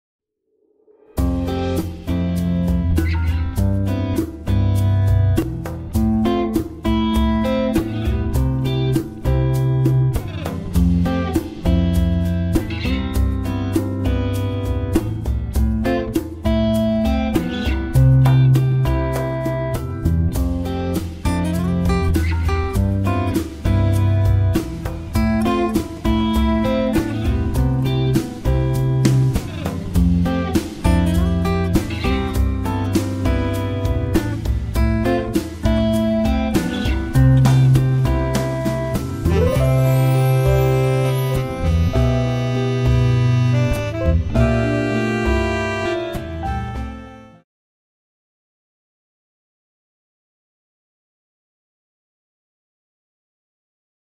Raido Tundra teardrop weekend trip
Weekend getaway to a free camp location in Norway with two of my nephews and the dogs, in the Raido Tundra teardrop camper.